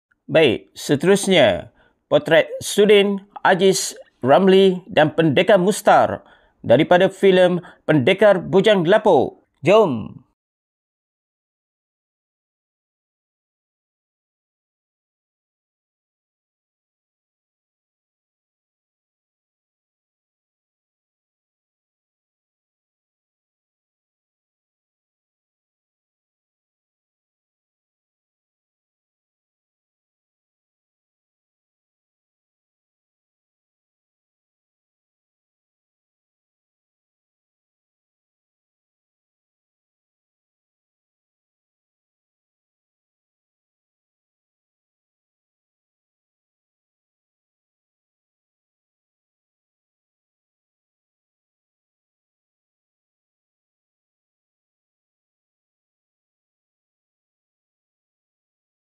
Cara Melukis Potret Watak Filem Pendekar Bujang Lapok Pakai Charcoal | Charcoal Drawing
Cara melukis potret watak filem Pendekar Bujang Lapok Sudin Ajis Ramli dan Pendekar Mustar pakai medium pensil charcoal